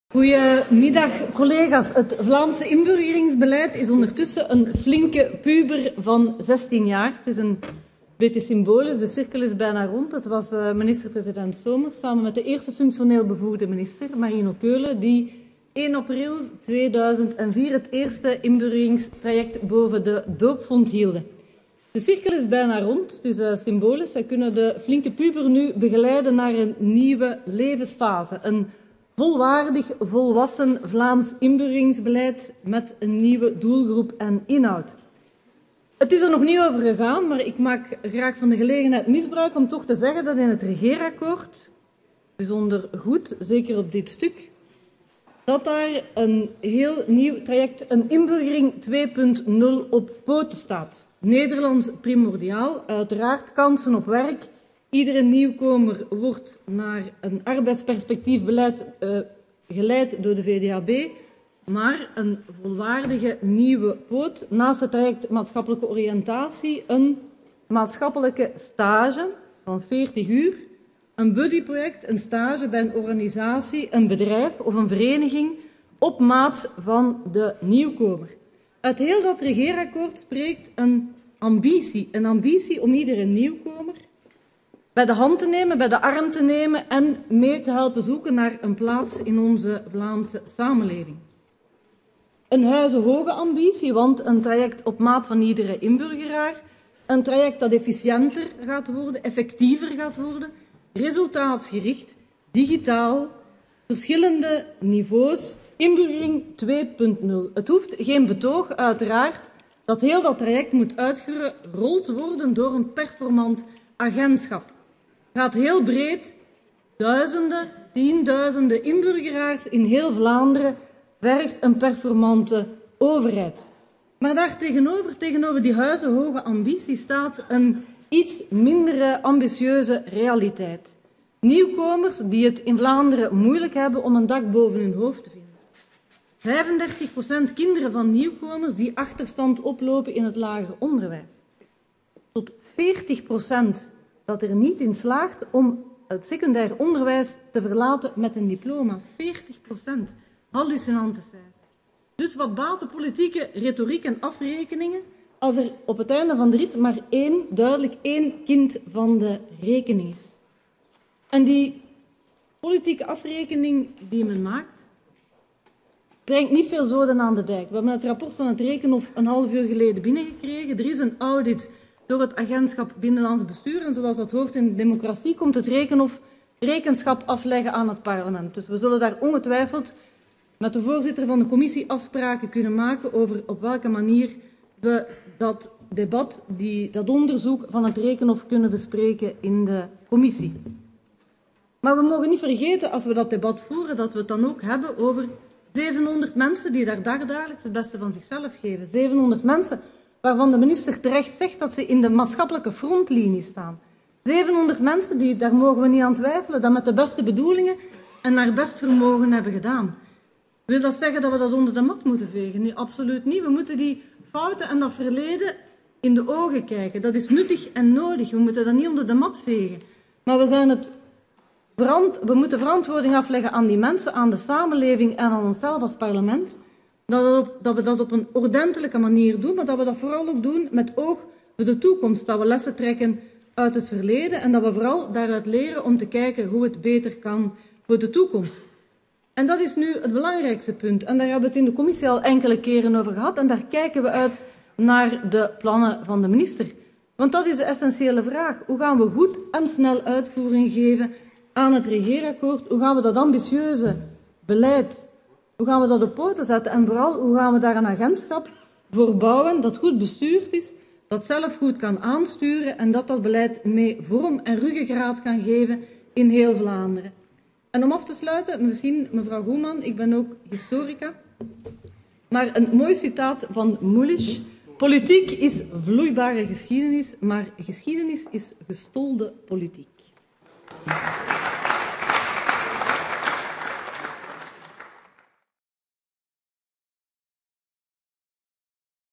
Inburgering 2.0 een noodzaak!
Katrien Partyka neemt deel aan het actua debat omtrent het nieuwe rapport van het Rekenhof dat ter beschikking is over de werking van Het Agentschap Inburgering.